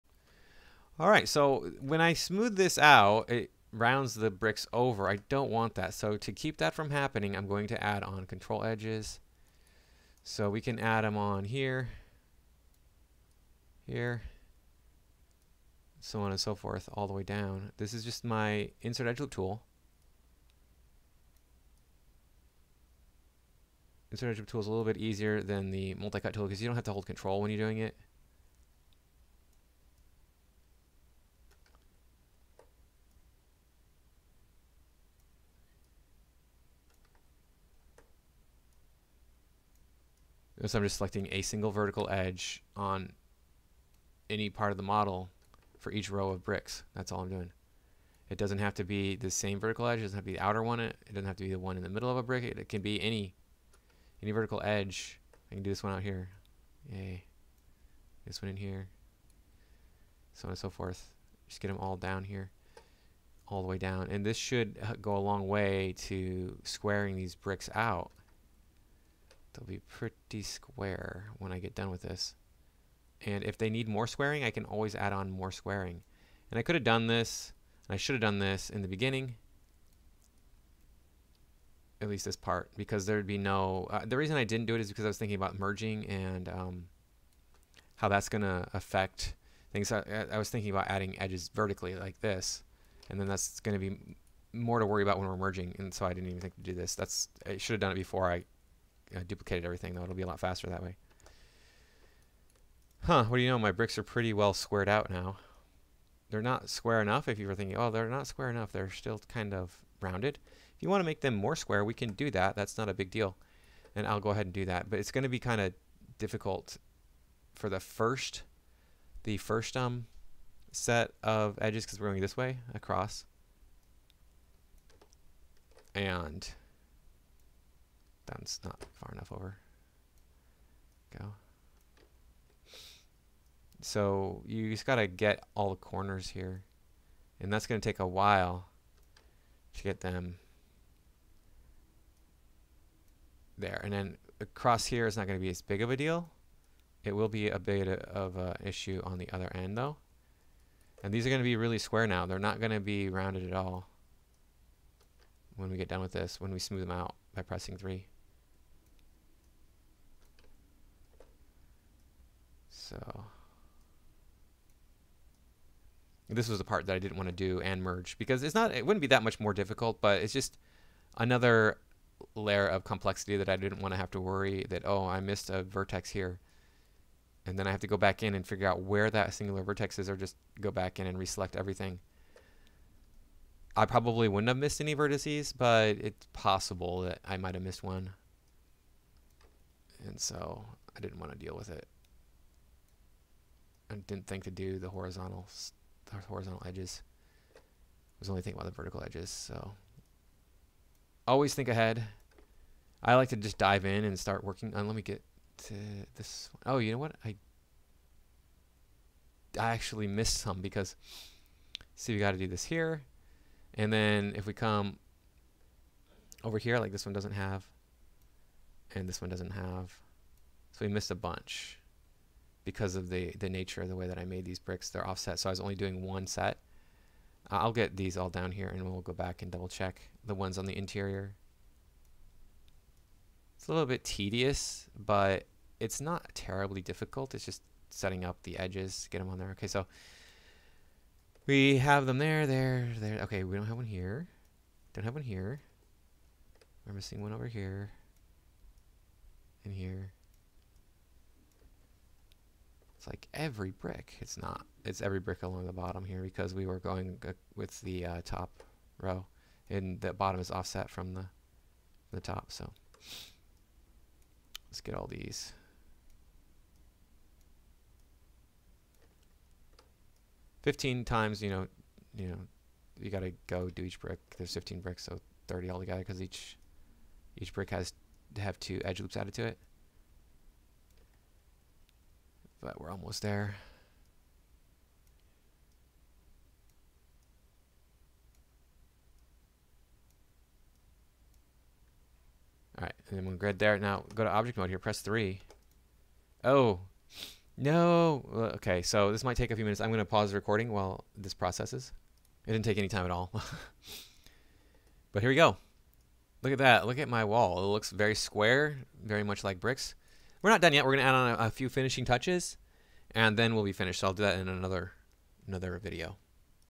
Modeling a Brick Wall in Autodesk Maya 2024, Part 3
In this video, I continue the process of modeling a brick wall in Autodesk Maya 2024.  I cover the adding control edges in this video.